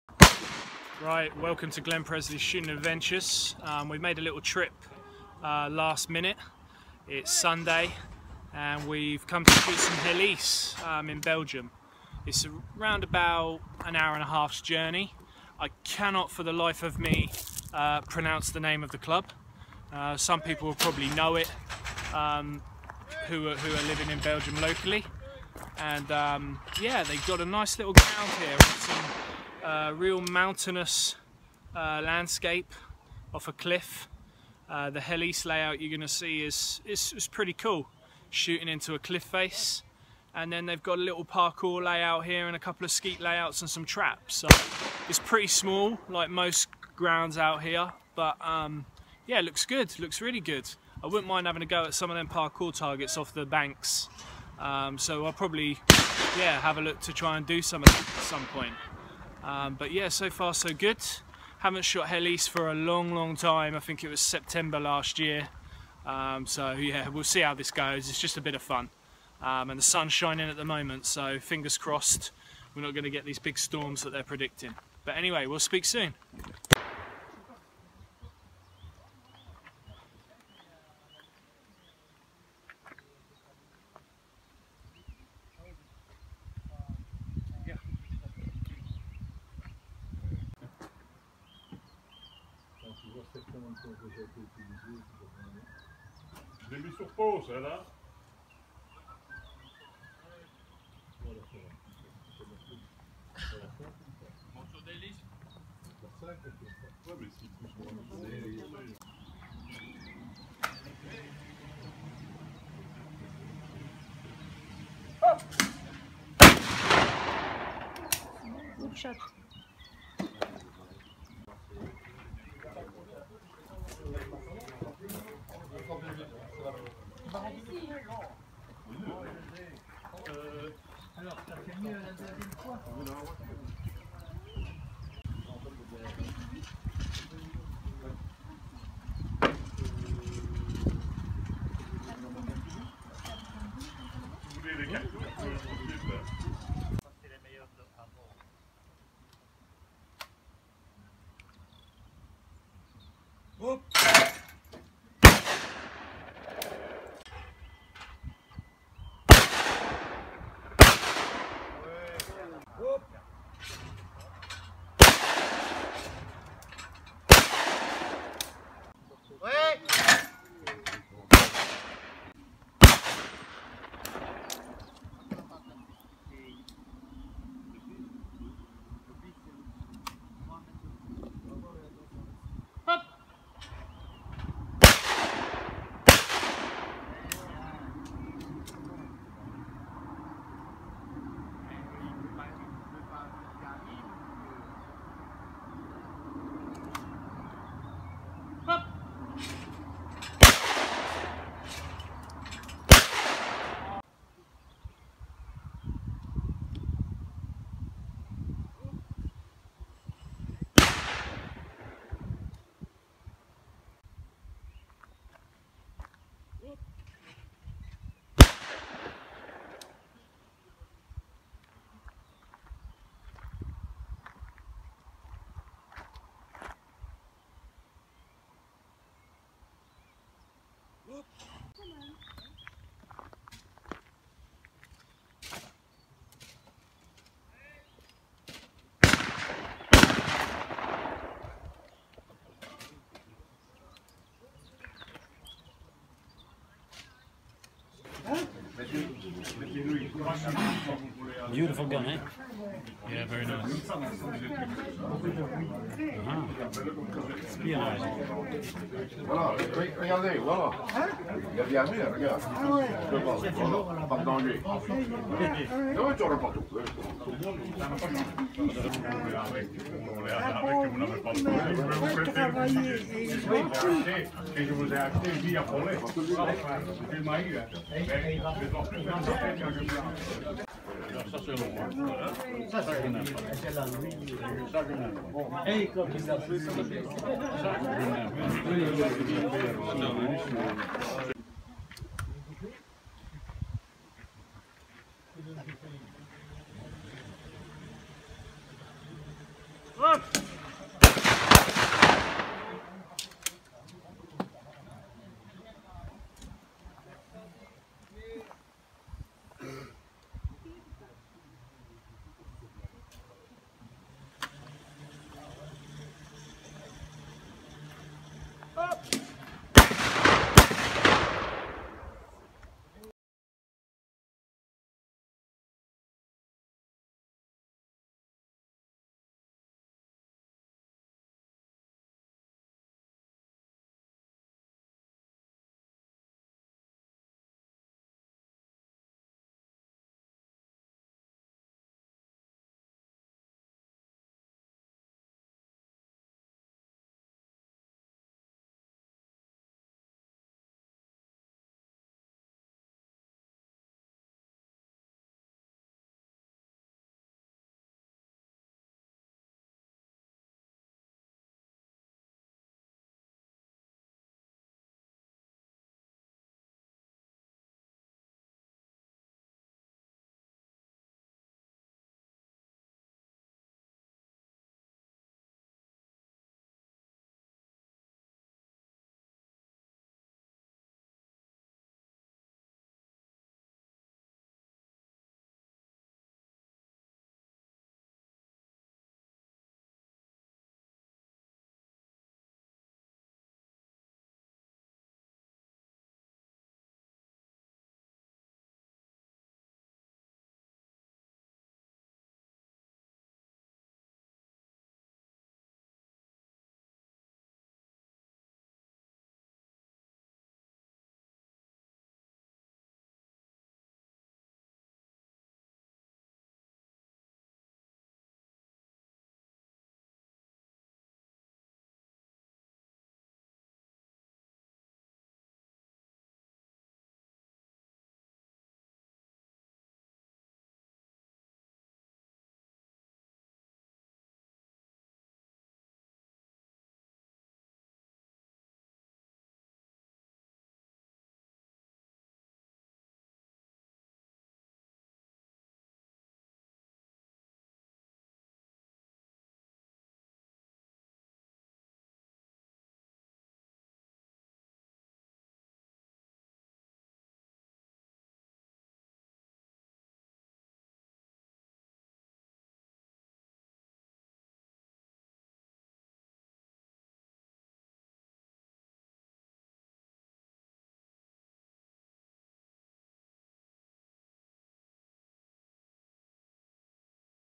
Glen Presley's Shooting Adventures 2016 - Episode 3, Belgian Helice 2016
Glen makes the trip to Dinant in Belgium at Clay Club Bouvignois for a spot of Helice shooting! Fun in the sun. and showers!